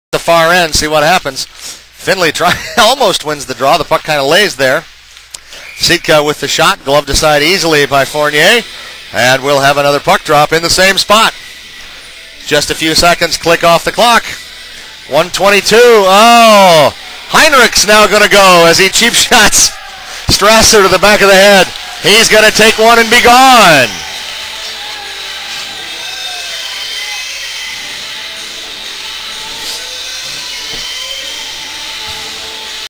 Strasser Gets a Cheap shot
Findlay Grrrowl 08-09

Hinrichs hits Strasser in the back of the head after the play stops.